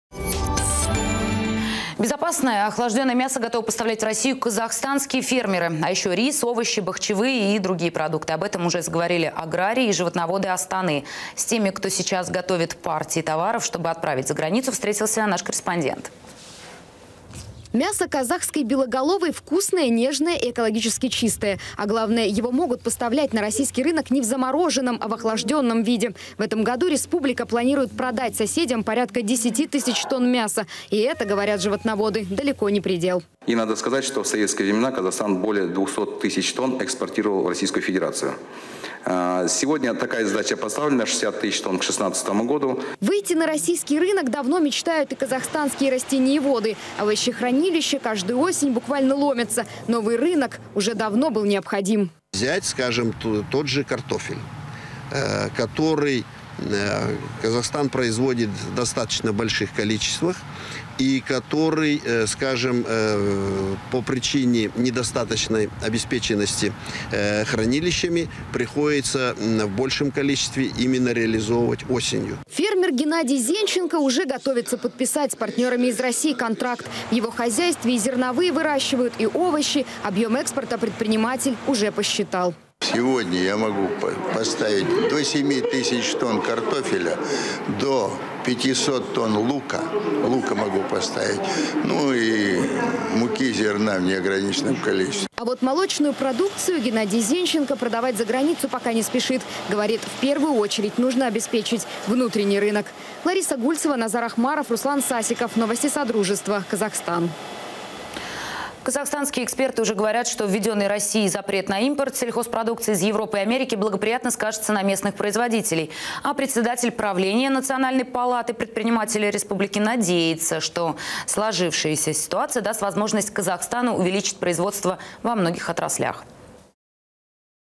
Казахстанские фермеры готовятся поставлять в Россию мясо и овощи
Казахстанские фермеры готовы поставлять в Россию охлажденное мясо, рис, овощи, бахчевые и другие продукты. Об этом уже заговорили аграрии и животноводы Астаны.